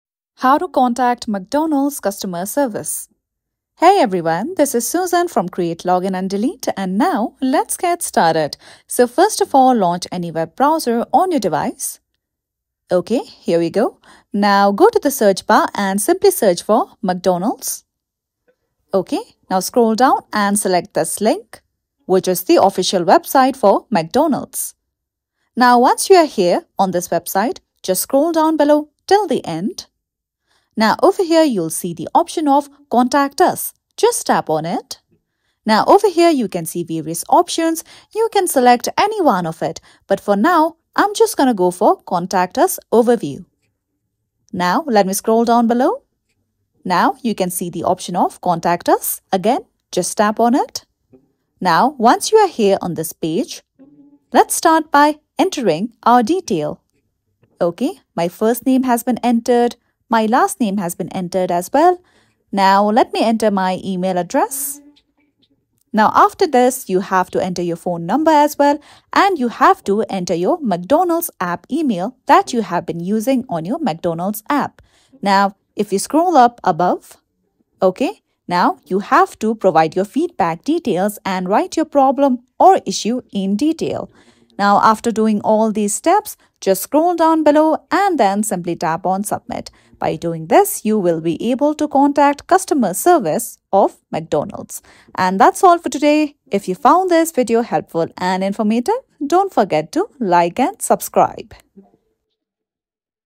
How To Contact McDonald's Customer Service (2024)
Tutorial On How To Contact McDonald's Customer Service

👋 Welcome to Create Login And Delete! In today's tutorial, we'll guide you through the process of contacting McDonald's customer service in 2024. If you have inquiries, feedback, or complaints, this video is designed to help you connect with McDonald's customer support efficiently.

Key topics covered in this tutorial:

1. **Contact McDonald's Customer Service:**
   Discover the various channels and methods to reach McDonald's customer service. We'll provide you with a step-by-step guide on how to get in touch with their support team for assistance.

   For those who prefer electronic communication, we'll explore how to contact McDonald's customer service via email. Learn the steps to compose and send an email to address your concerns or inquiries.

3. **McDonald's Head Office Complaints:**
   If you have significant concerns and wish to escalate your matter, we'll guide you on how to file complaints with McDonald's head office. Understand the process and steps to ensure your feedback is heard at the corporate level.

📱 Timestamps:

0:00 - Introduction
0:05 - How To Contact McDonald's Customer Service